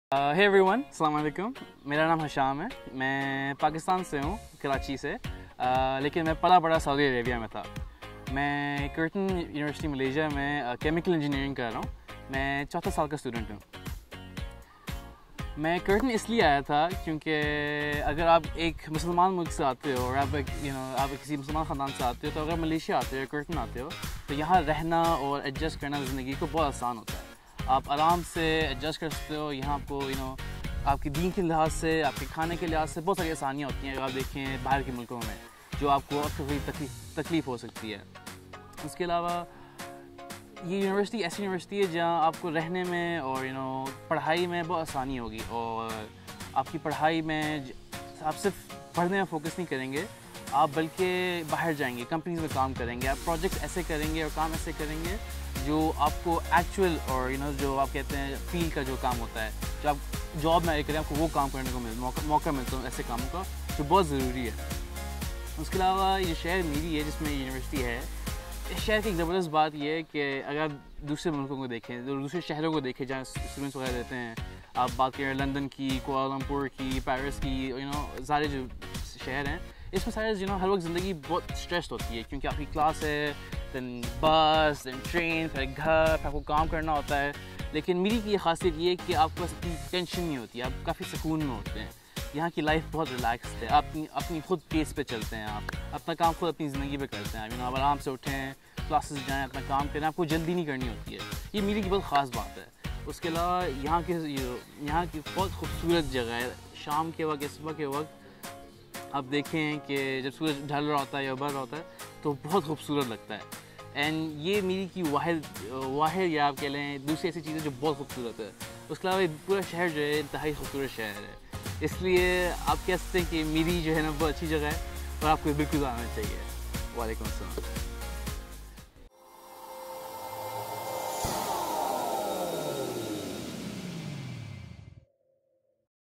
Adjusting to life at Curtin Malaysia
Meet Hasham, an international student from Pakistan who found home here at Curtin Malaysia.